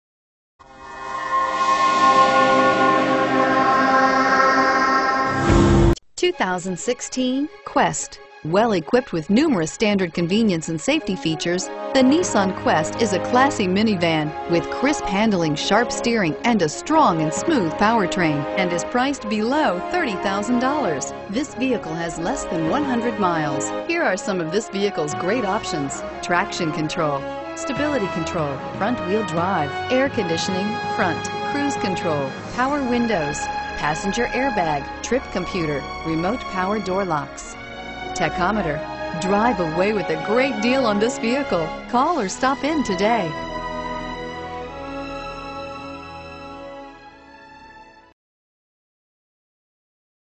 2016 Nissan Quest Bedford, Fort Worth, Dallas, Arlington, Hurst TX N17210
Grubbs Nissan
310 Airport Fwy

You'll love this 2016 Nissan Quest. This is a  you'll want to take home. With 2 miles, it features Automatic transmission and an exterior color of Brilliant Silver.